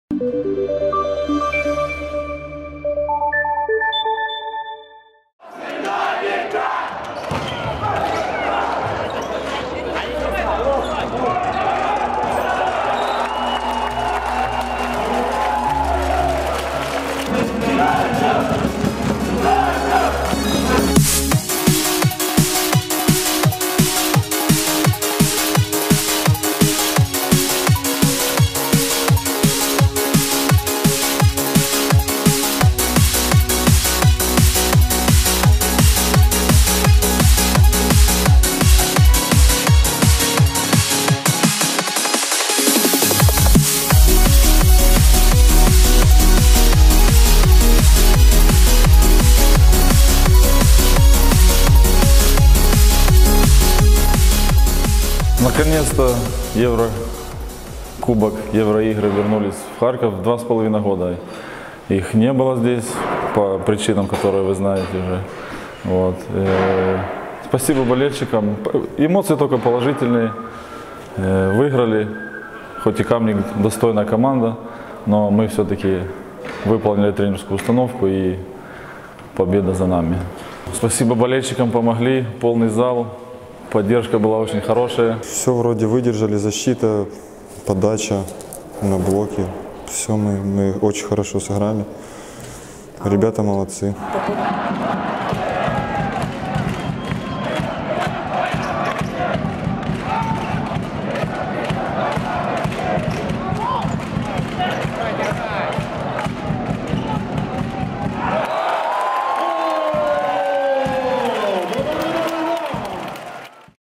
Харьковский "Локомотив" победил в первом матче Европейского Кубка Вызова
В первом волейбольном матче 1/8 Финала Кубка Вызова между командами "Локомотив" (Харьков) и "Кальцит" (Камник, Словения) со счетом 3:1 победили харьковчане.

Напомним, что впервые за 2 года европейская конфедерация волейбола позволила харьковскому "Локомотиву" провести домашний еврокубковый матч в Харькове.